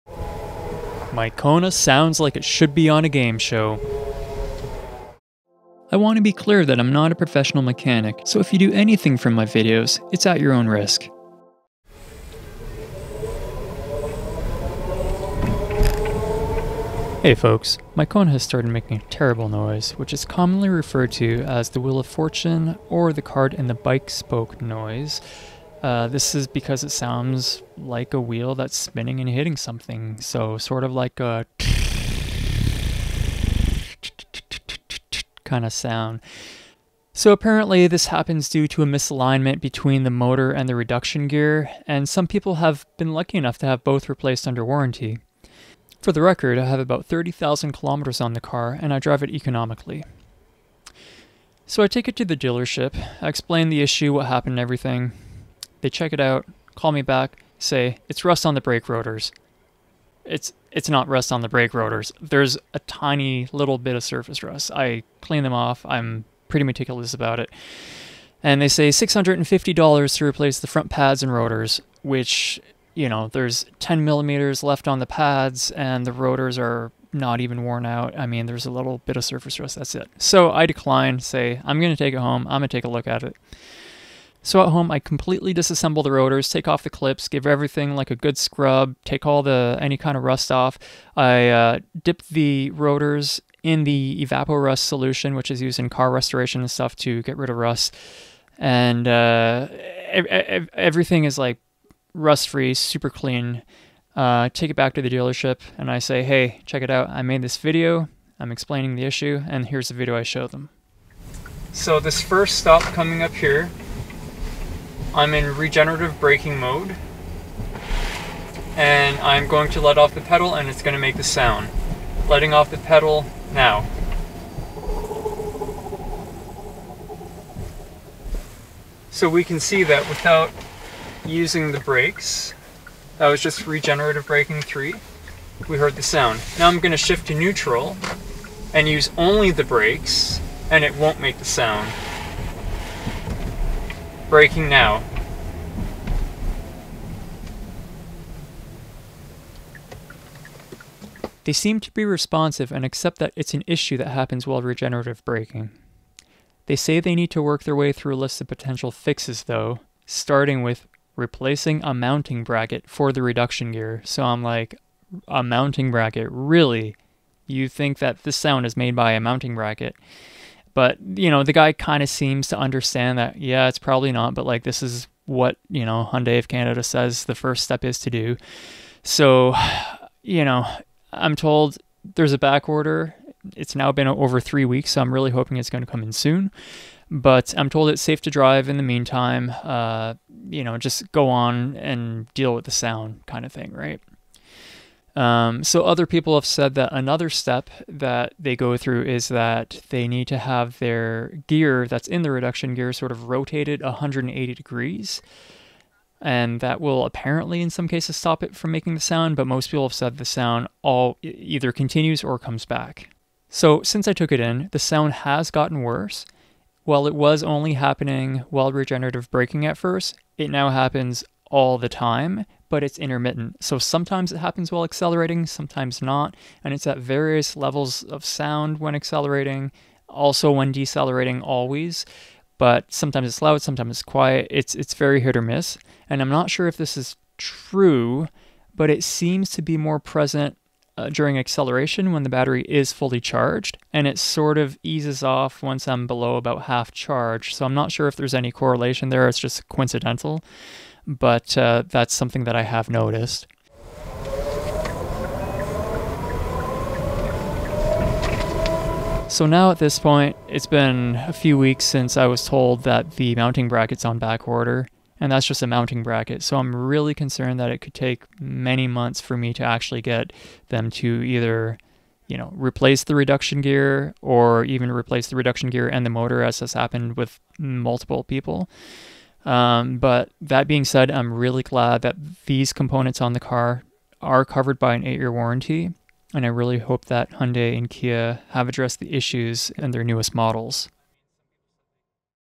Kona EV - Time for a new motor & reduction gear at 30,000kms?
UPDATE DOWN LOWER!
My Kona EV has started making the dreaded "Wheel of Fortune" or "Card in the Bike Spoke" sound. It started very suddenly one day. I was driving down highway 11 in Ontario, approaching the 400, and when I let off the accelerator pedal for the bend in the road (regen kicking in), the car started making an awful screeching sound as if it were driving on a rim. I knew it wasn't a blown tire though, cause the car was handling fine and the air pressure was fine, and after pulling off the highway and checking the shield behind the front wheels, I ruled out a rock that was stuck in there. The rest of my experience, so far, is in the video.

0:00 Intro
0:13 The noise
0:51 Dealership experience
1:50 Proving it wasn't the brakes
2:36 Steps they need to take
4:13 In closing

Update! I got my reduction gear and traction motor replaced today. Here's my timeline.

Feb 13 - The noise started very suddenly, and VERY loud, while driving on the highway.

Feb 16 - The dealership had told me I needed new front brake pads and rotors. I declined.

March 8 - After spending my time taking them all apart and cleaning them to prove them wrong...(sigh...), they acknowledge that there was an issue and ordered a mounting bracket (lol) to try to fix the issue. In their defence, this is what Hyundai of Canada says is the first step that must be taken.

April 6 - Mounting bracket changed and, big surprise, the noise remains! Both the traction motor and reduction gear failed rumble test. Parts ordered.

May 18 - Parts replaced. Hurray!

All things considered, everything was replaced much faster than I thought it would be. I've heard of people driving their cars around like this for a year, and someone told me they heard of someone's reduction gear exploding, though I don't have anything to verify that, so it could be false.

I'll eventually make another video, but will be quite busy for the next month or so, but thought I'd at least keep you folks in the loop. Cheers